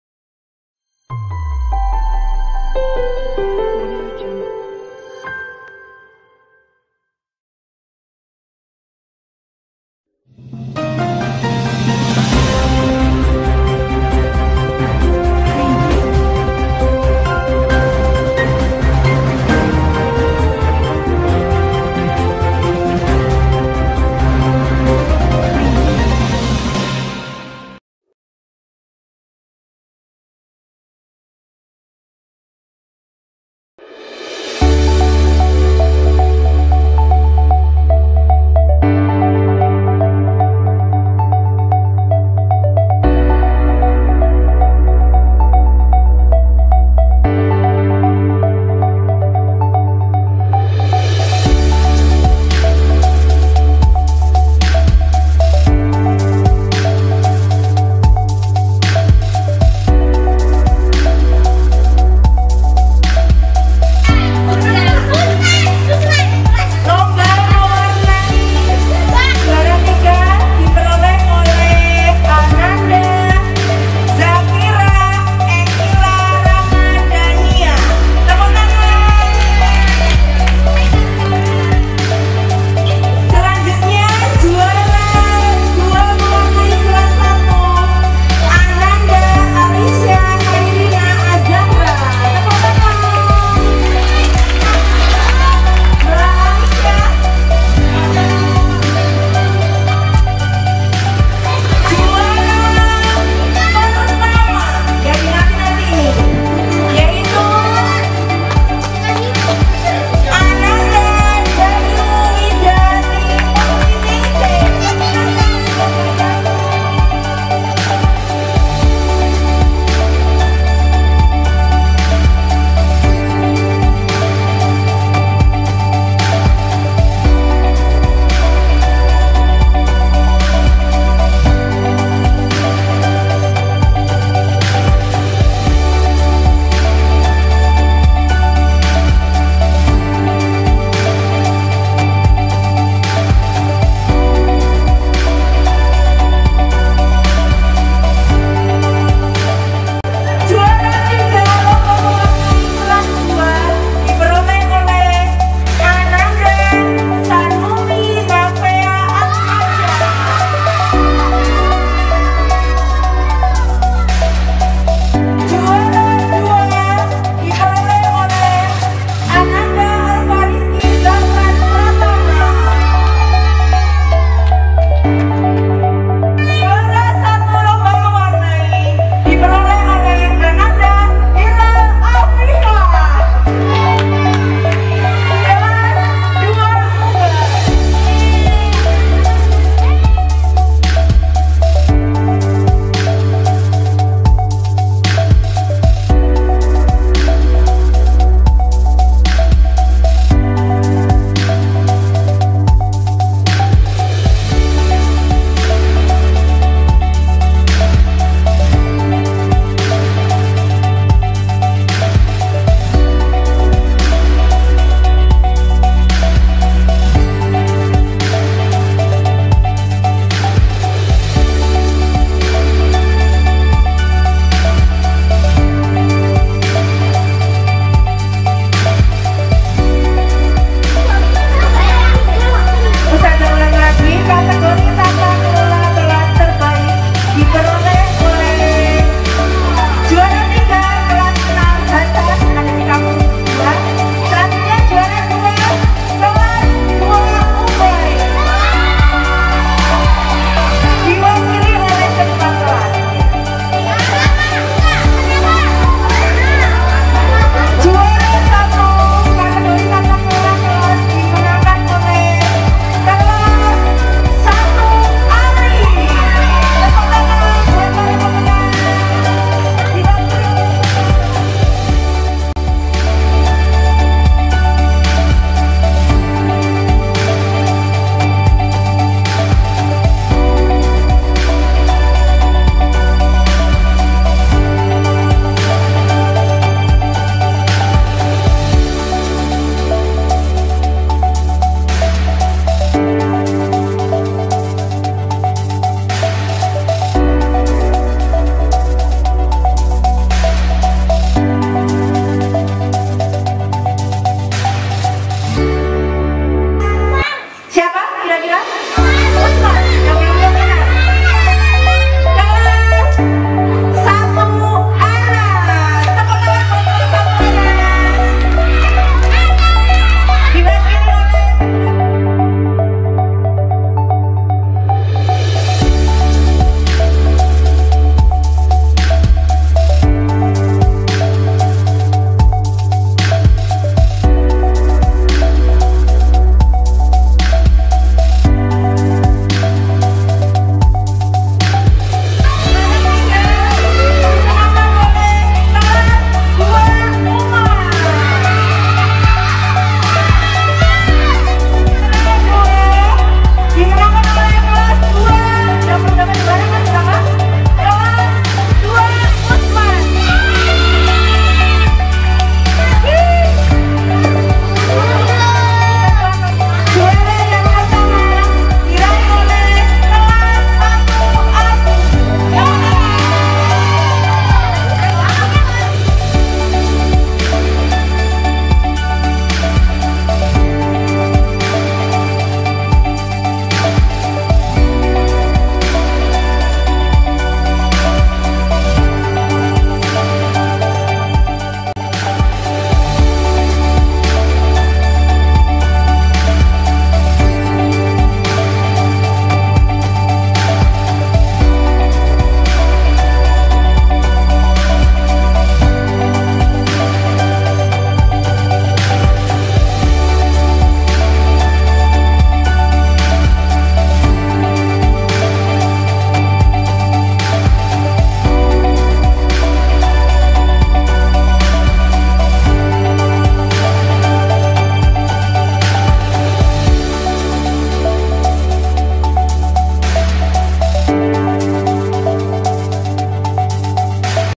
Breaking News!!!: "Pengumuman Juara Lomba Hut RI ke-77" (Kampus 1)
Assalamu'alaikumsahabat viralku...

Berikut kami sampaikan Juara Lomba dalam rangka memperingati HUT-RI ke 77 dan menyambut muktamar ke-48

Penasaran...? yuk simak videonya sampai habis...

Wassalamu'alaikum...